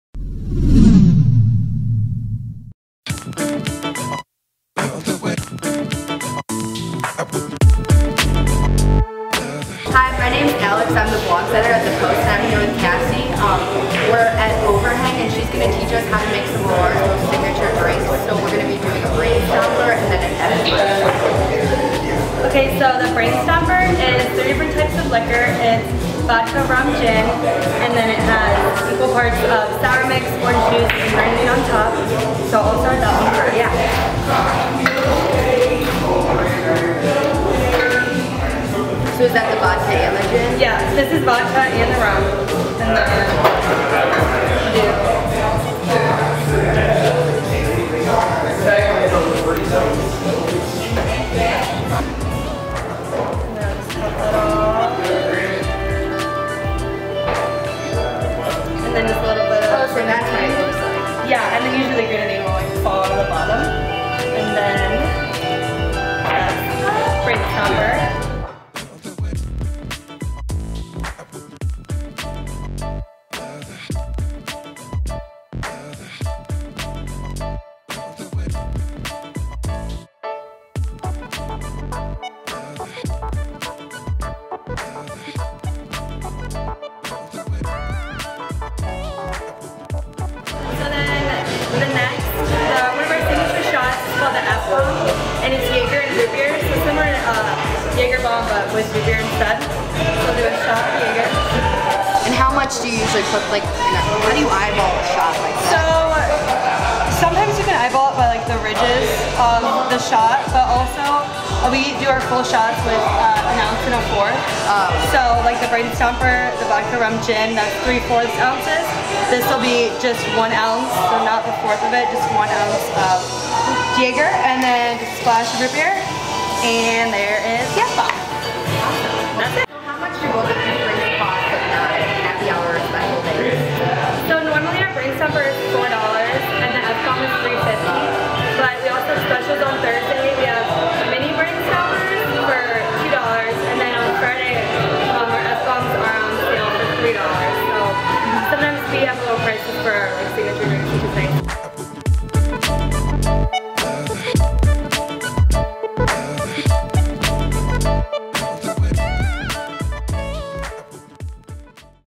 Signature Drinks - Overhang
Alex Darus heads to Overhang to learn how the bartenders there make a Brainstomper and an F-Bomb shot. Overhang is located at 63 N Court St.

Tabloid print edition out every Thursday.

Talent - Alex Darus
Editor - Alex Penrose
Film - Alex Penrose and Andrew T. Hamilton